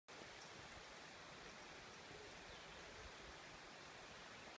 Emaline running through Joyce Kilmer
The best part of my day was running along with Emaline!